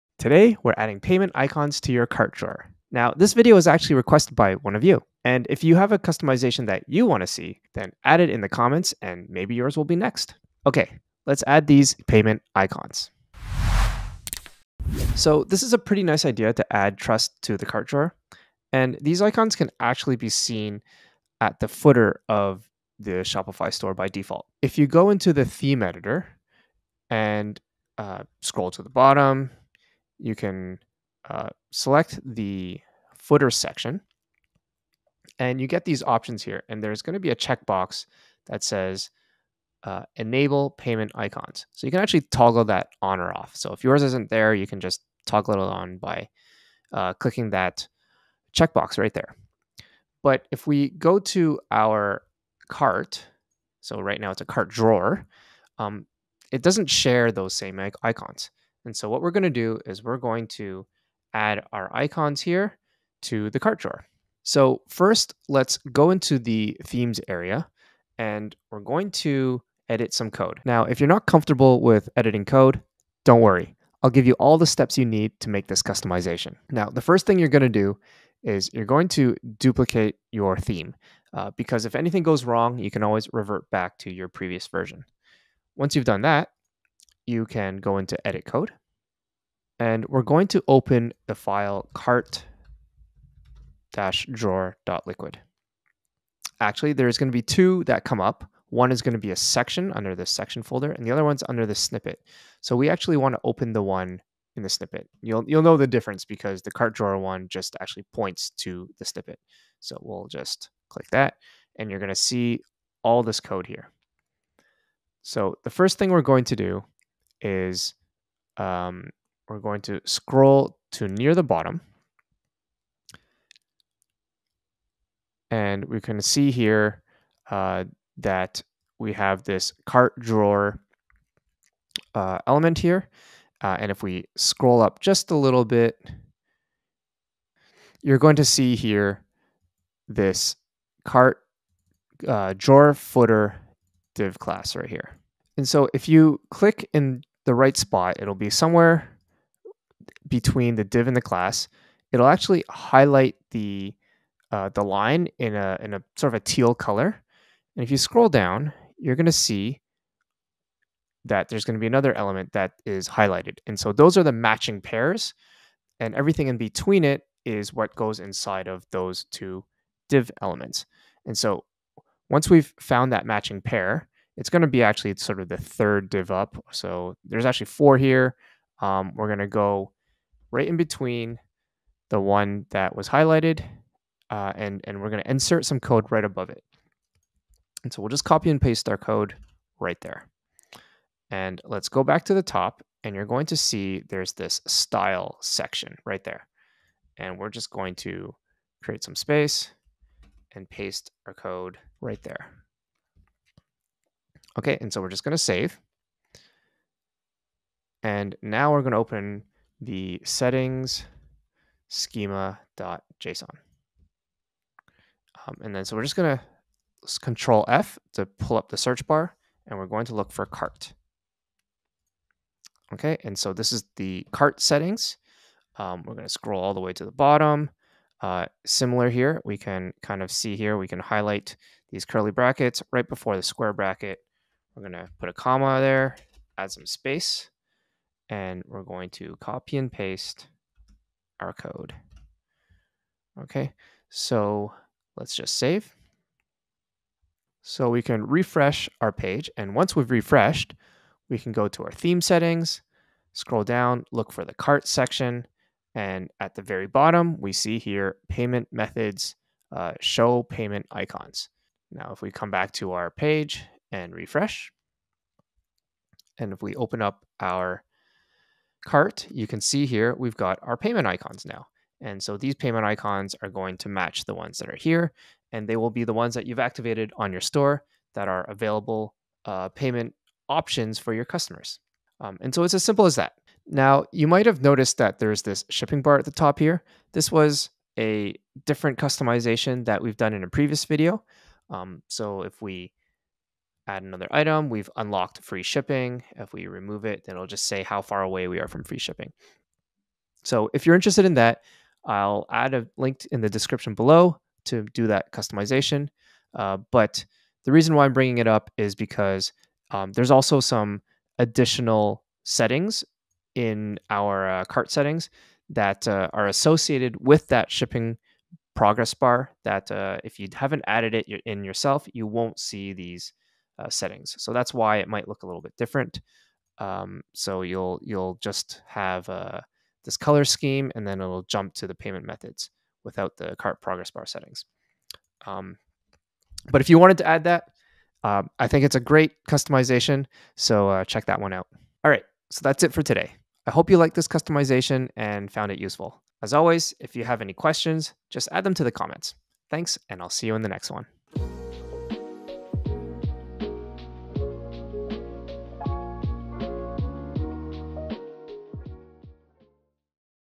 How to Add Payment Icons To Your Cart Drawer (add trust)
Code for this video can be found here when you join our free newsletter

The payment icons can be shown on your shop's footer (we show you how in case you wanted to know), but in the cart drawer that's not an option that is available. This customization shows you how you can add it under your checkout button.

Don't worry if you're not yet comfortable with code, all the steps you need are in this video.

TIMESTAMPS
00:00 - Intro
00:15 - How to add or remove the payment icons to your shop's footer
01:27 - Edit code and theme editor settings to add the payment icons
04:25 - Demo of the icons on the live store
05:42 - Final thoughts

Themes: Dawn, Refresh, Craft, Studio, Publisher, Crave, Origin, Taste, Colorblock, Sense, Ride, Spotlight